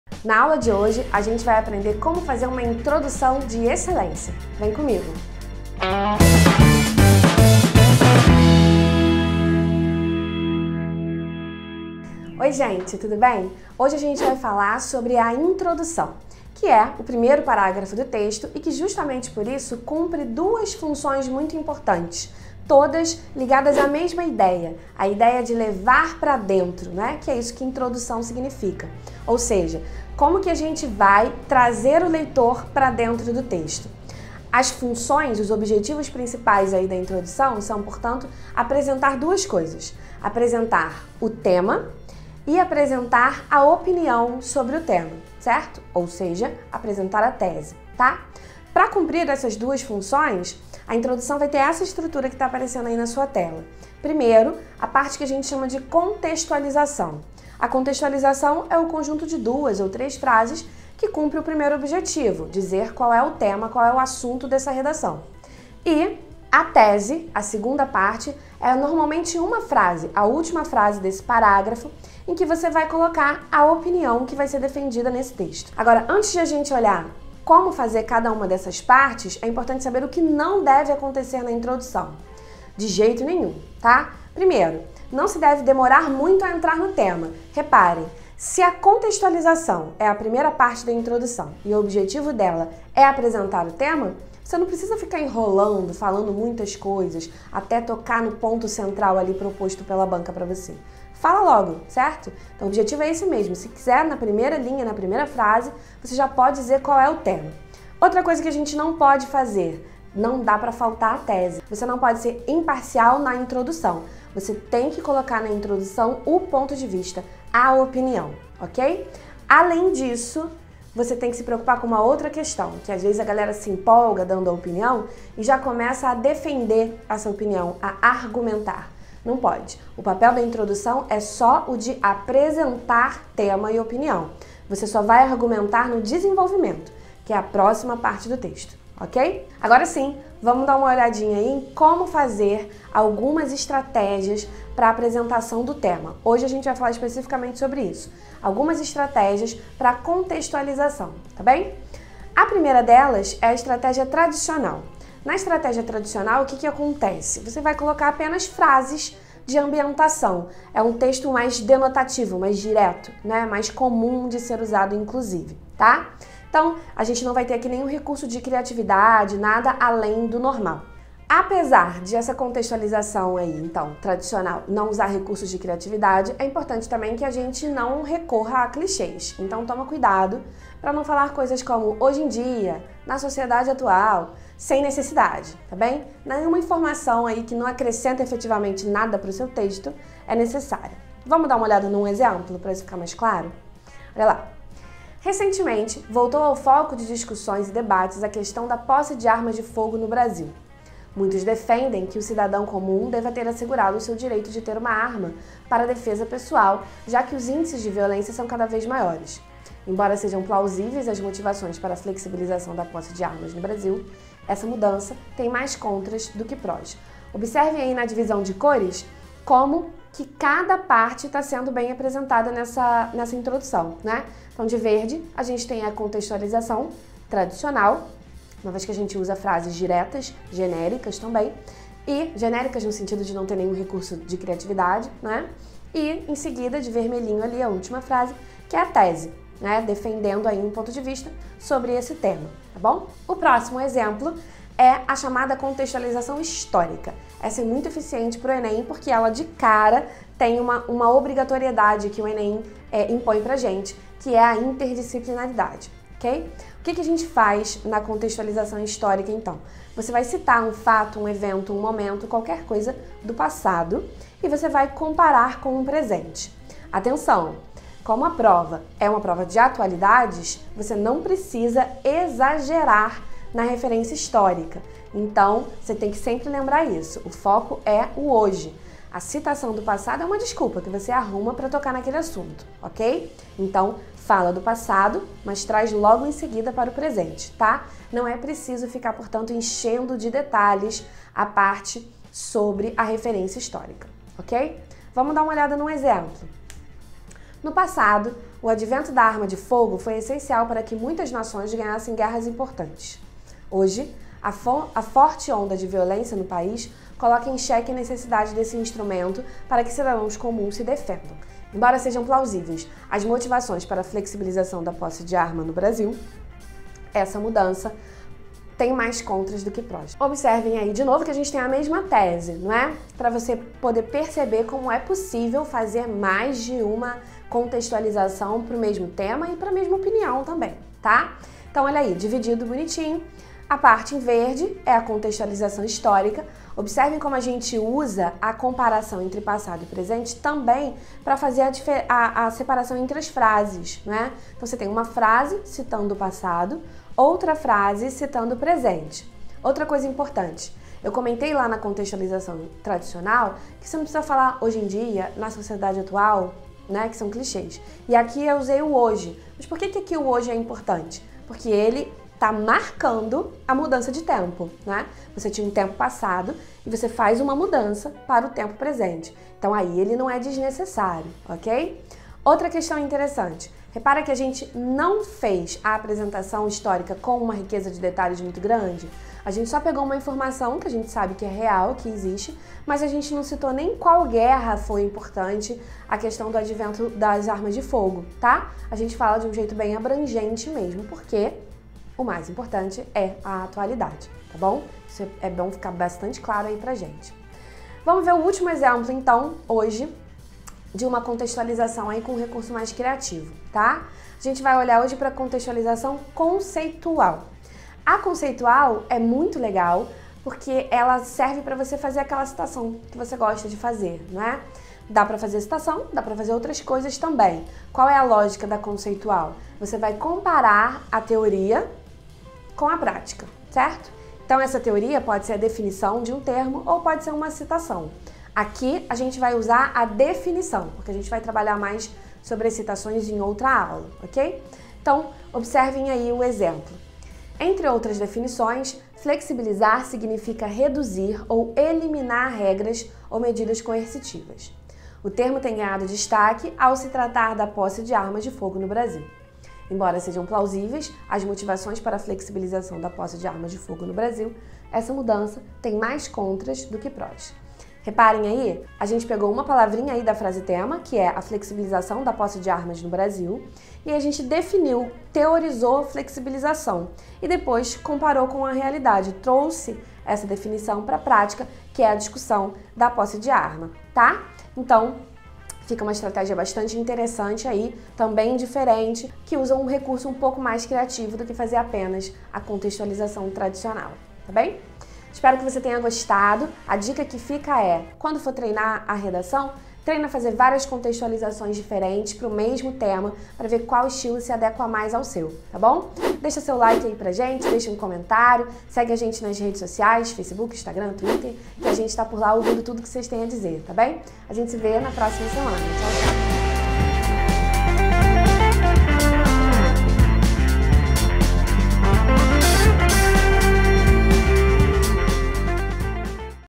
INTRODUÇÃO NOTA MÁXIMA | Oficina Textual | Agora, Disserte!
Nossas redes:
A primeira impressão é a que fica? Nem sempre. Mas na redação do vestibular uma boa introdução pode ajudar e muito. Na aula de hoje, a professora Helena Faria te mostra 3 estratégias de contextualização para você arrasar.